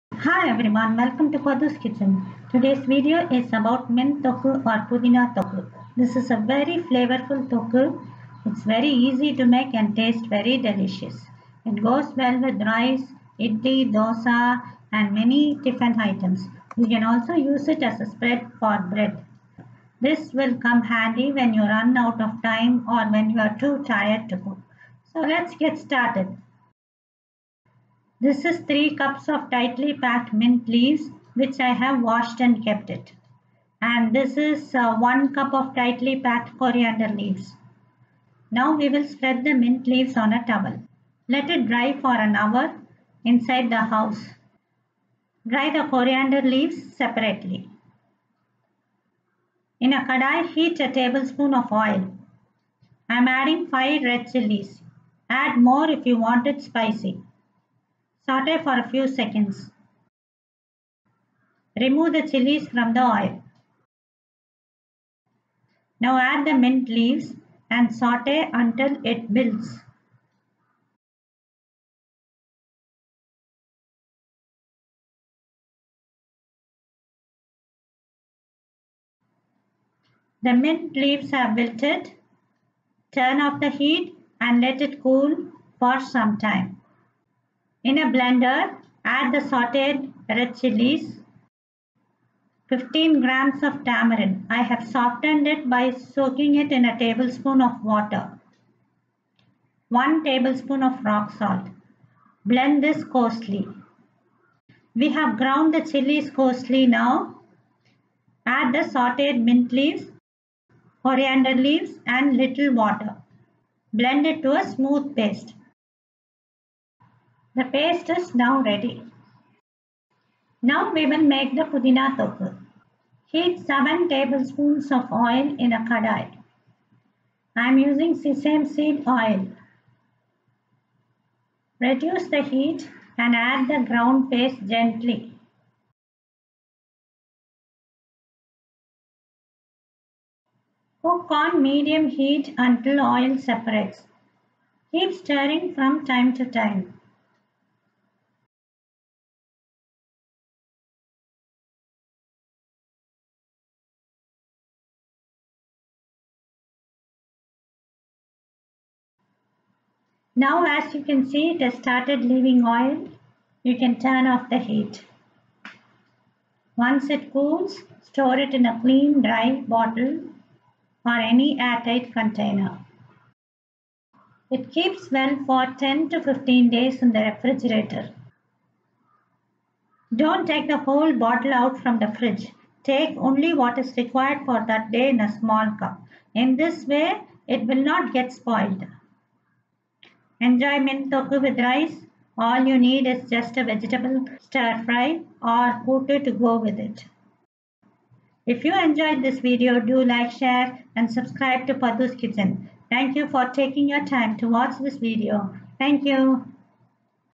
Mint Thokku Recipe-Keeps well for 15 days-Pudina Thokku
Pudina Thokku-Mint Thokku is very flavorful, easy to make and very delicious. It goes well with rice, idli, dosa, parathas, chapati, adai or can also be used as a spread for bread. Thokkus come handy when you run out of time, when you cannot cook an elaborate meal or too tired to cook. Do try this simple and flavorful mint thokku and leave your feedback in the comment section.

Detailed Recipe in my website Padhuskitchen.com